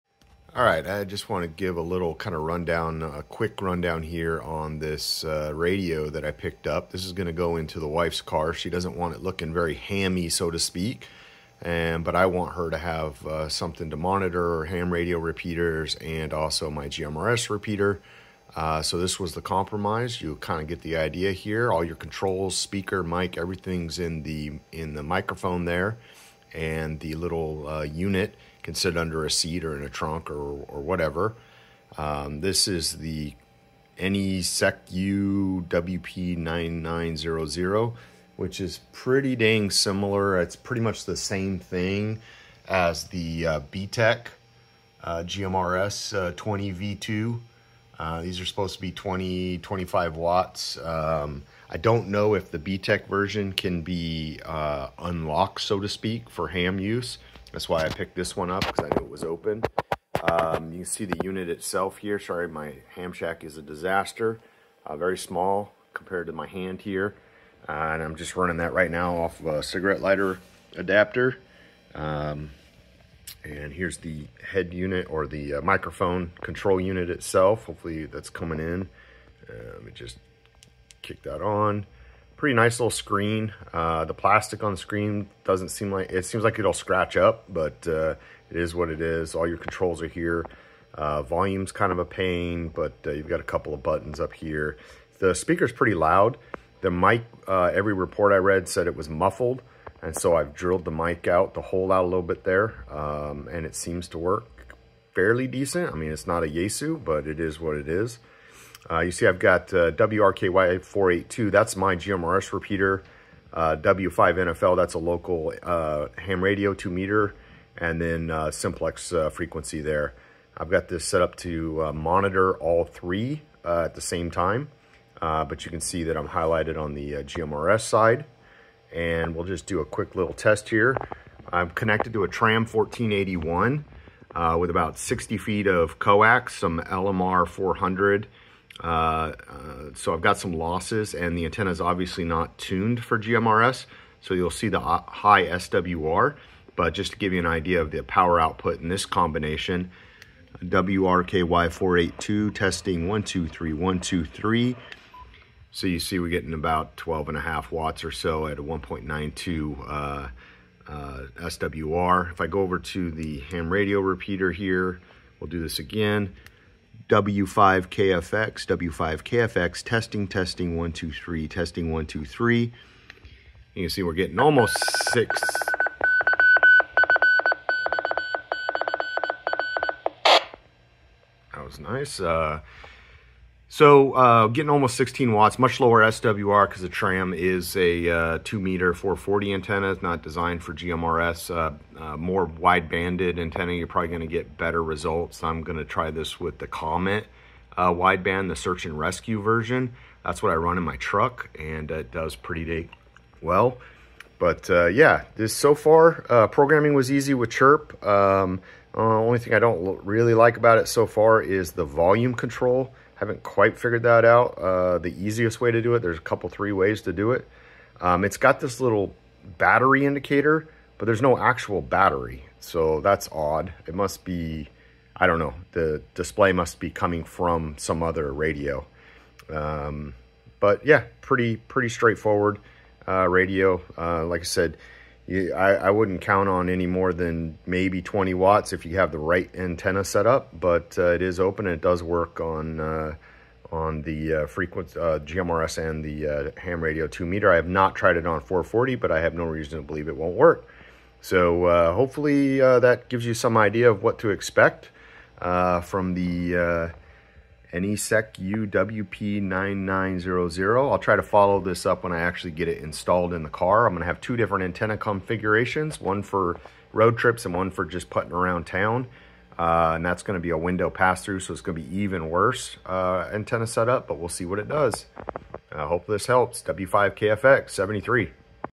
Anysecu WP-9900 "Mini Radio" HAM & GMRS test & first impression
Test this 20w open radio for power output and first impressions. Similar or the same as the BTECH GMRS-20v2